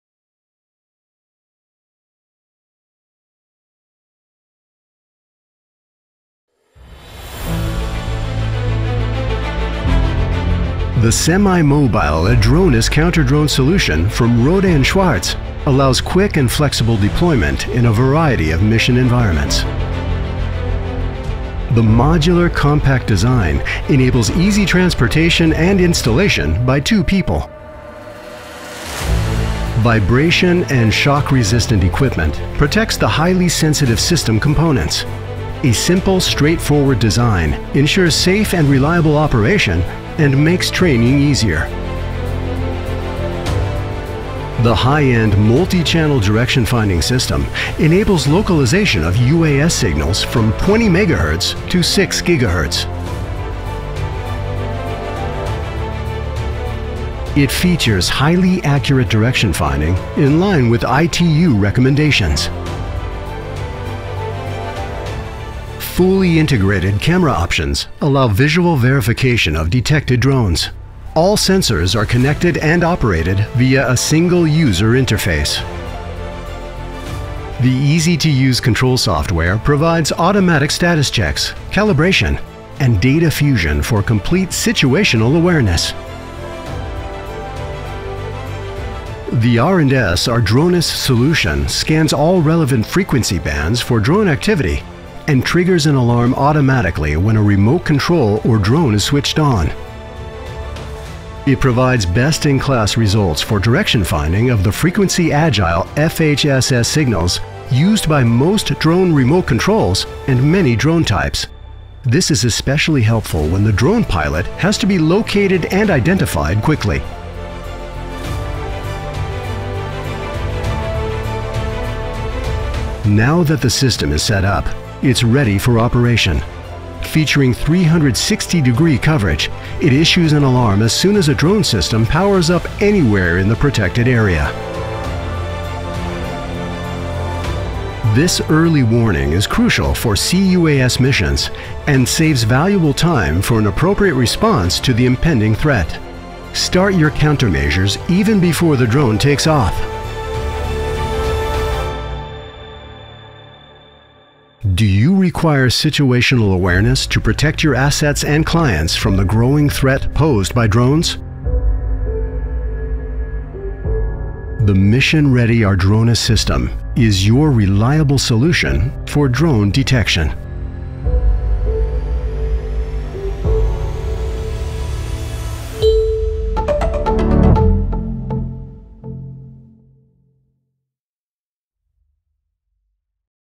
R&S®ARDRONIS LOCATE ADVANCED - Getting mission ready
When R&S®ARDRONIS detects commercial drone activity, it automatically classifies the type of drone signal, determines the direction of the drone and its pilot, and disrupts the radio control link to prevent the drone from reaching its target.